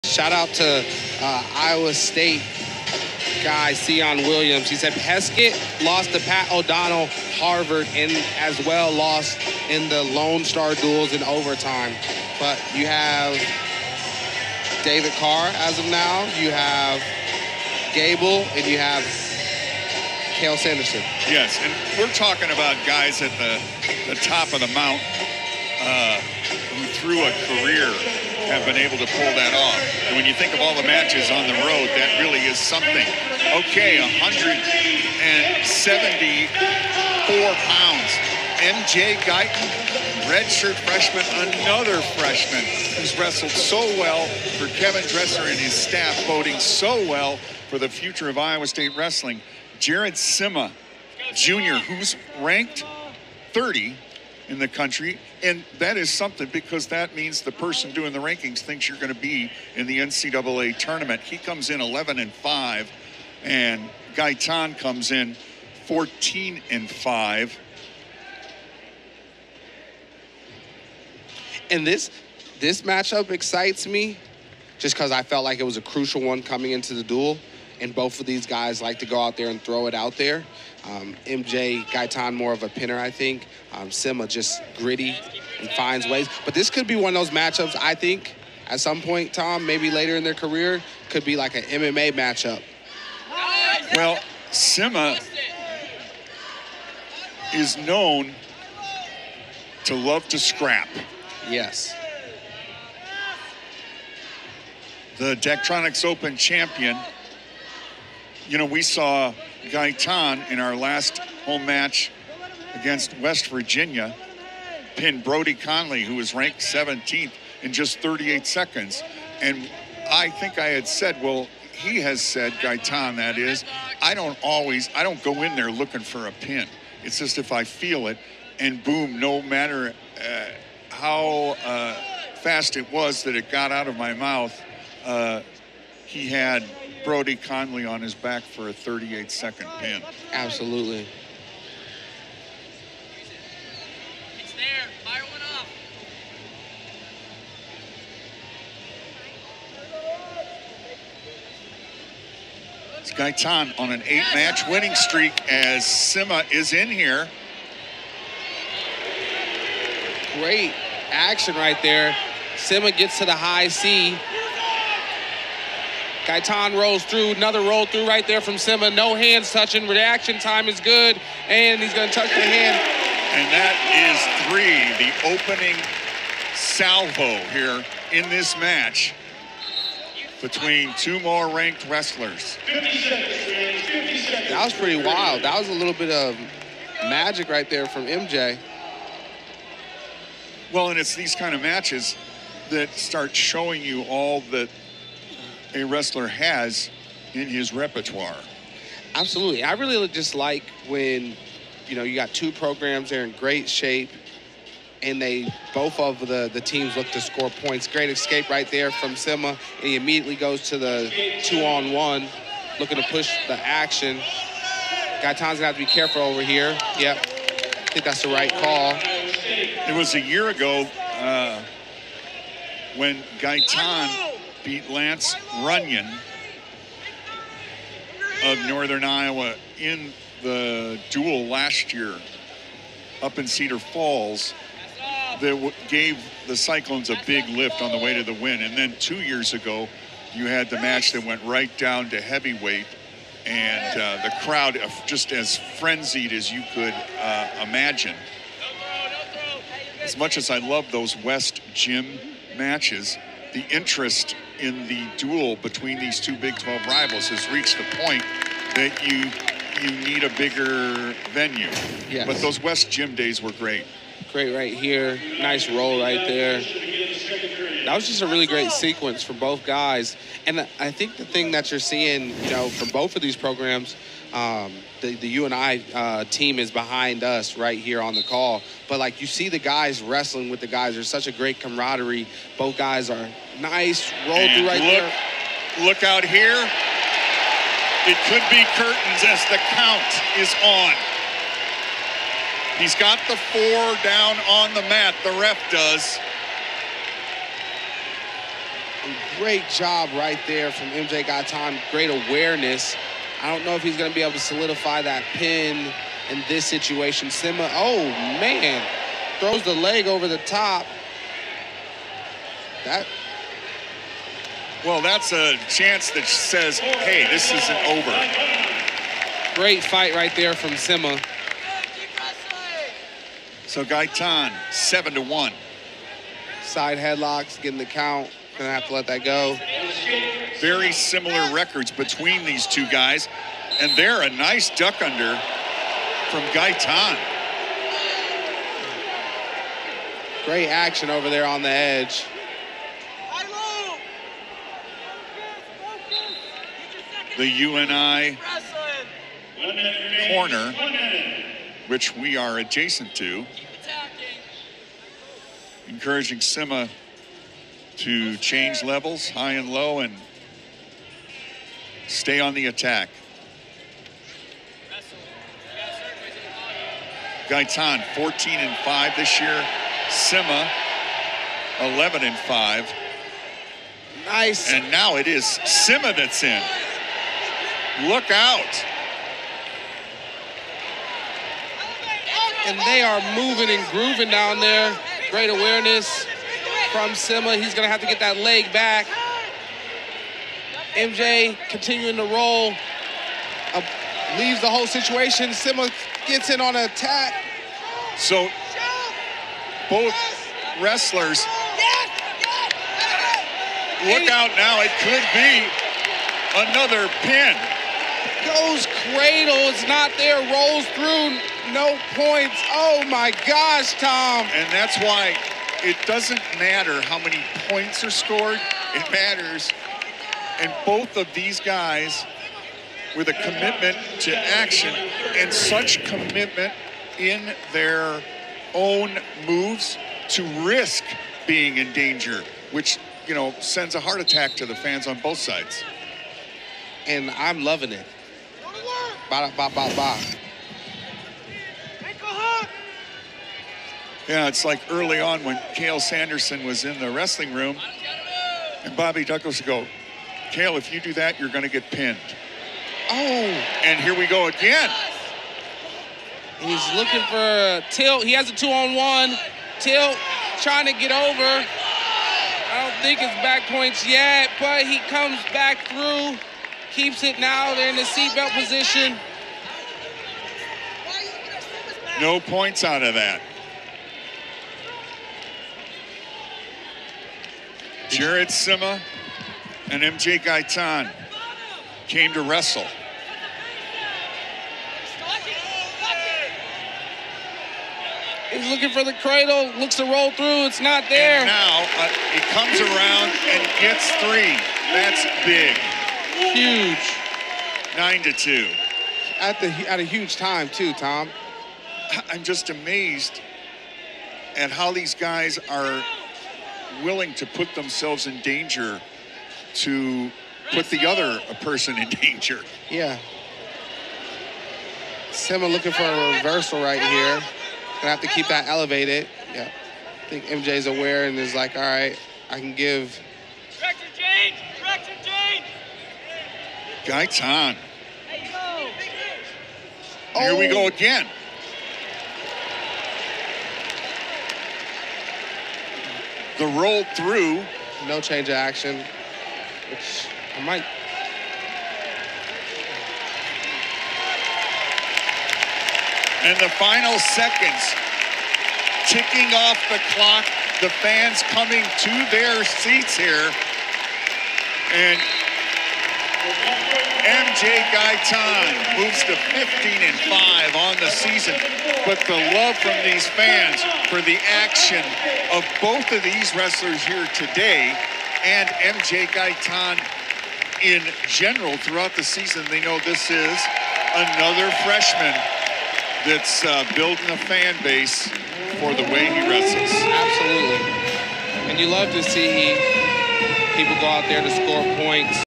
174lbs MJ Gaitan (Iowa State) vs Jared Simma (UNI)
2024 Iowa State vs UNI wrestling dual
174lbs #19 MJ Gaitan (ISU) maj dec #30 Jared Simma (UNI)